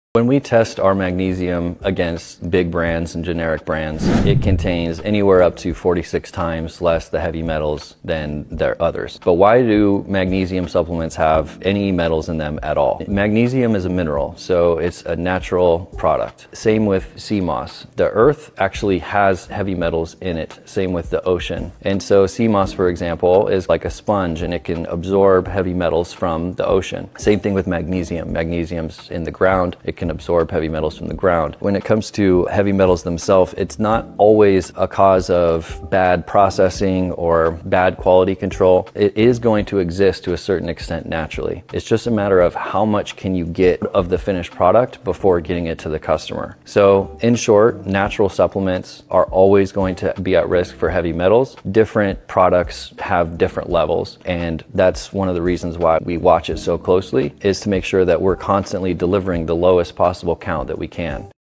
Why Does Your Magnesium Have Heavy Metals In It?
🔬 Why Heavy Metals Exist in Natural Supplements 🔬

At Organics Ocean, we test our magnesium against big and generic brands—and it contains up to 46 times less heavy metals. But why do magnesium supplements contain any metals at all? 🤔

Magnesium is a mineral, a natural product. Just like sea moss, it comes from the Earth and the ocean—both of which contain naturally occurring heavy metals. 🌍🌊

Sea moss acts like a sponge, absorbing heavy metals from the ocean. Similarly, magnesium in the ground can absorb metals from the soil. So while it’s natural, heavy metals aren’t always a sign of bad processing or quality control—they’re just part of the environment.

The real question is: How much can we remove to ensure the cleanest product possible for you? And that’s why we focus so heavily on keeping the heavy metal count as low as possible in every batch we deliver.

We also always publicly share our test results on our website, so you can see the purity for yourself. 🔍✅

In short, natural supplements will always carry some risk of heavy metals. But at Organics Ocean, we monitor it closely to ensure you get the highest quality product, with the lowest possible metal count.